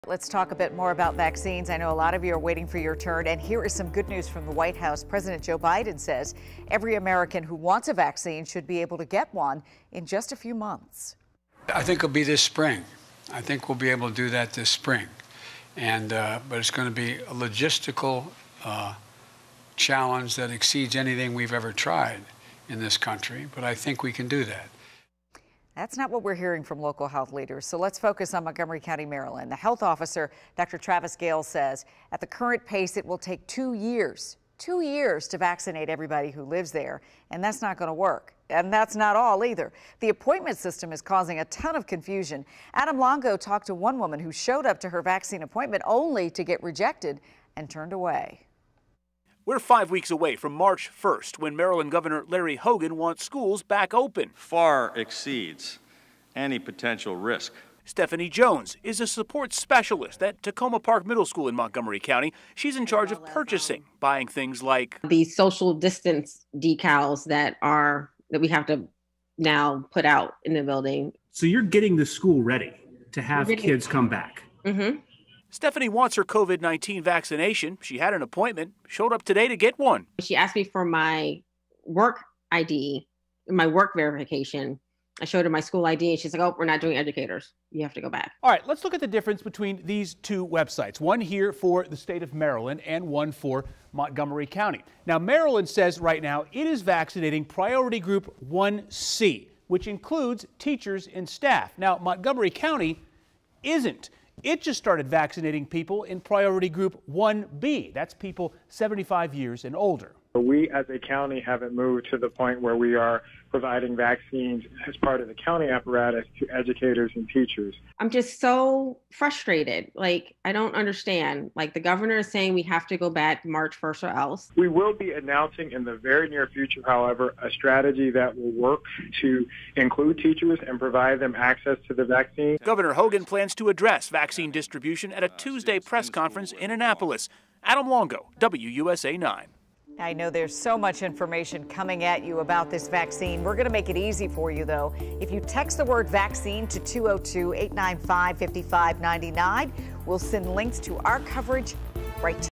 Maryland vaccine appointments: Some being turned away
Teachers and school staff are making appointments for a vaccine but getting turned away.